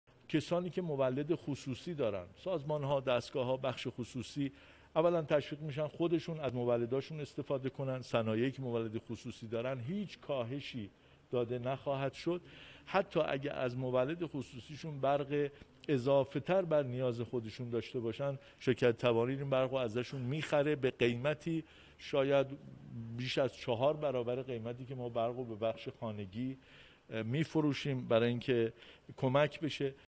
خرید برق مولد های خصوصی 4برابر قیمت بخش خانگی
خرید برق مولد‌های خصوصی ۴ برابر قیمت بخش خانگی
وزیر نیرو گفت: وزارت نیرو برق تولیدی مولد‌های خصوصی را تا ۴ برابر قیمت برق عرضه شده به بخش خانگی، خریداری خواهد کرد.